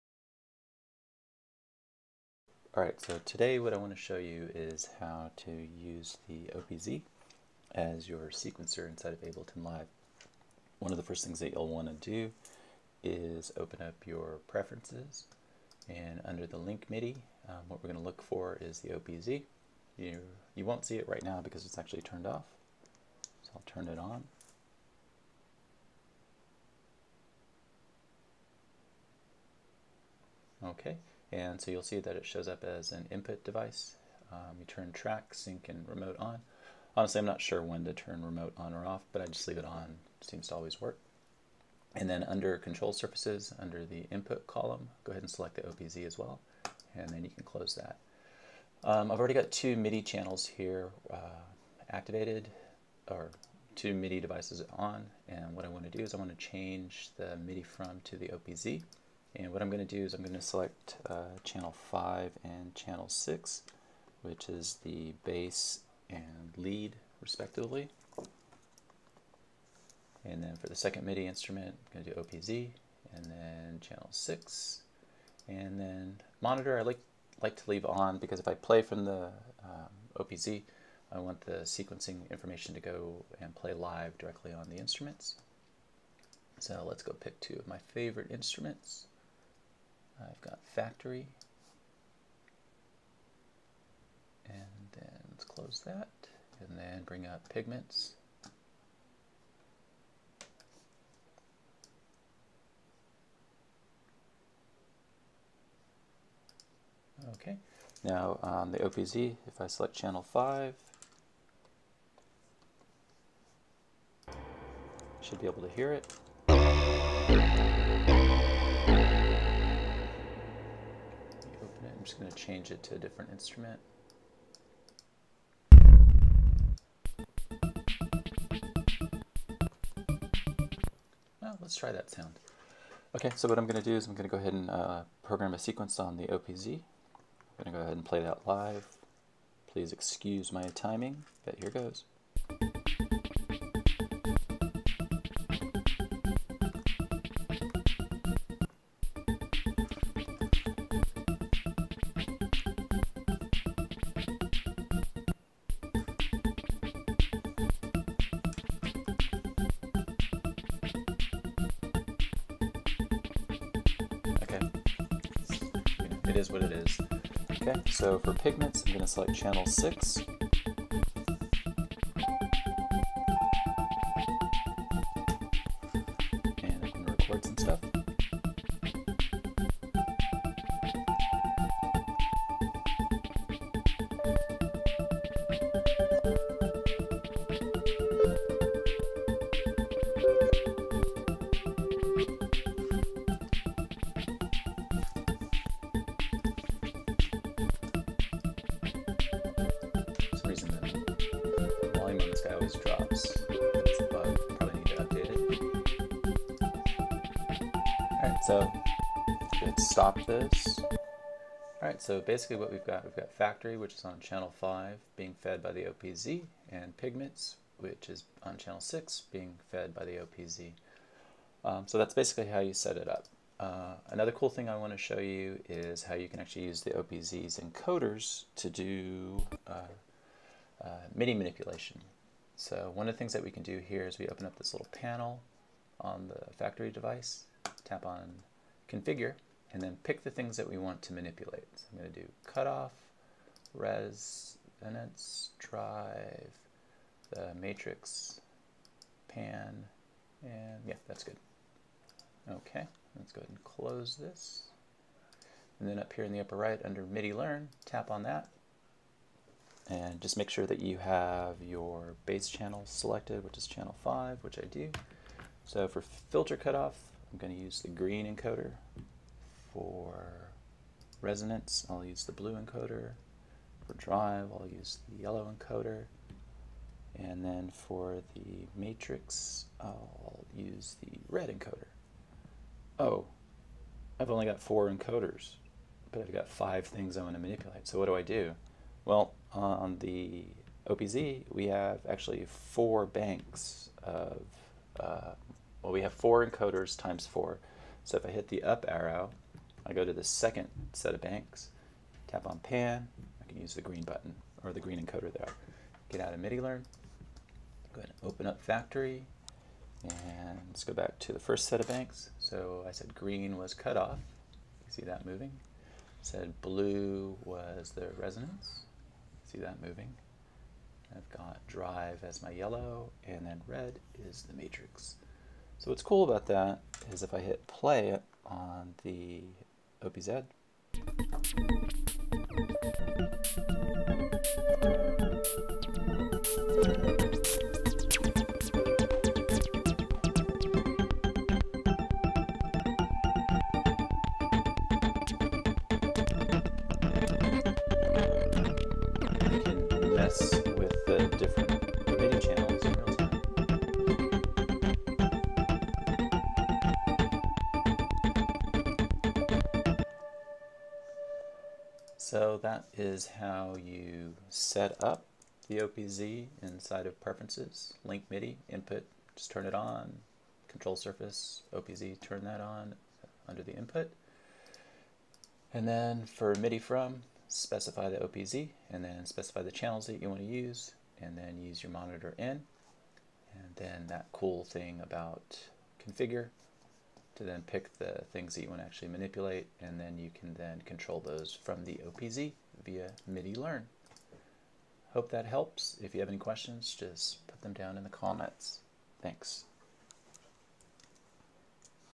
OP-Z + Ableton Sequencing Setup
A quick overview of how to set up the OP-Z to be used as a midi sequencer in Ableton Live.